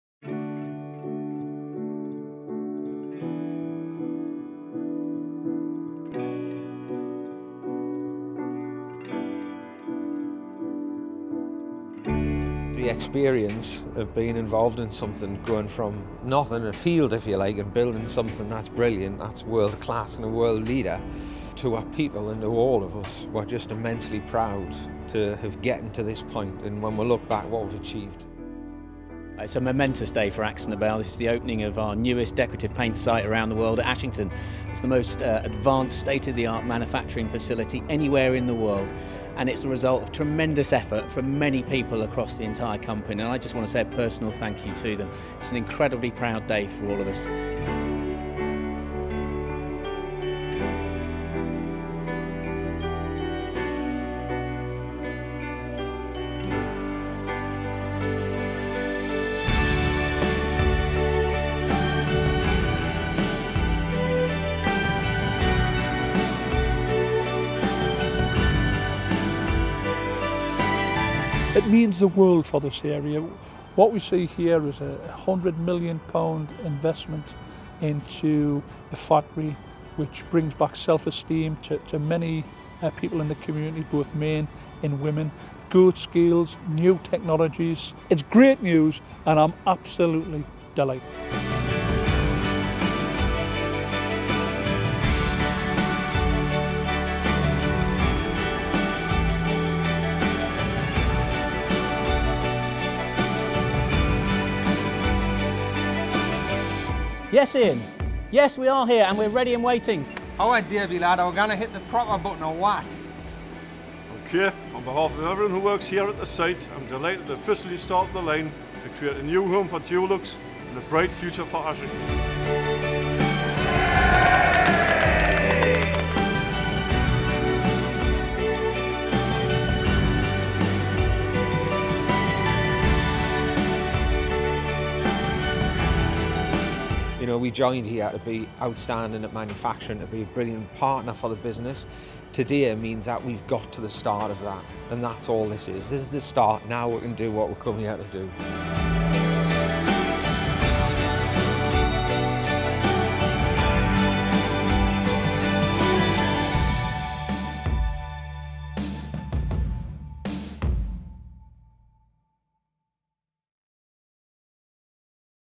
Ashington Highlights
AkzoNobel has officially opened the world’s most advanced and sustainable paint plant in Ashington, in the UK. This video has all the highlights of the opening.